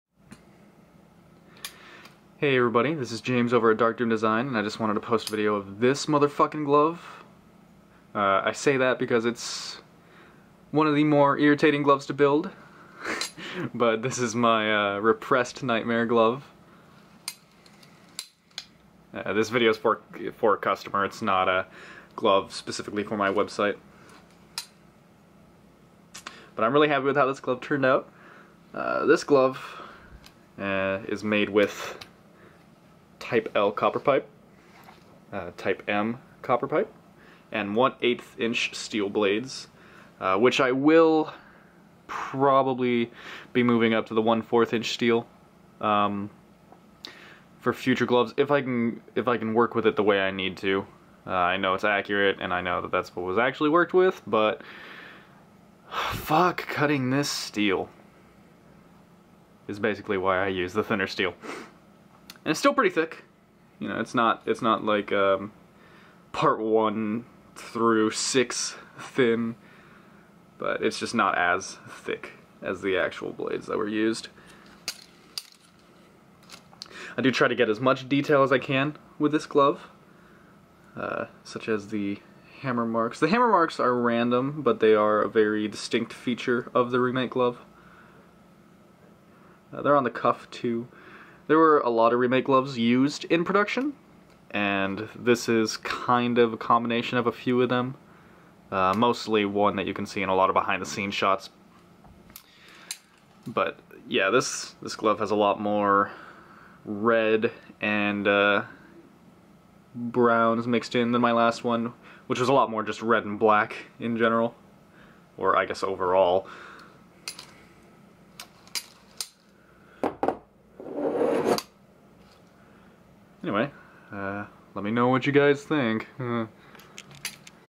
New - Repressed Nightmare
My newest Repressed Nightmare glove
Made with Type M and L copper pipe, 1/8 inch steel blades Mig welded on, and a suede Keystone leather glove.

$280+shipping